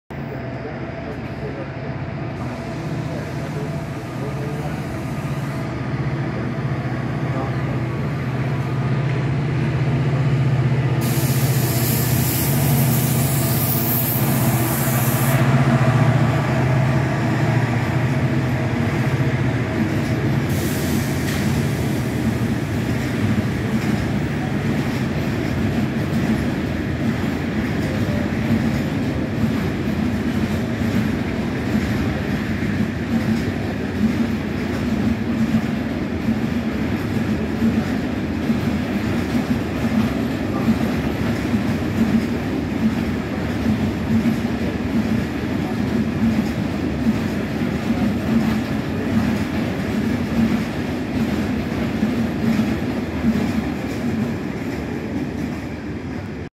335 de transfesa pasando por vicalvaro el día 16-9-21 martes a las 22:32 minutos va al sur
😁🚂🇪🇦🚦video realizado en vicalvaro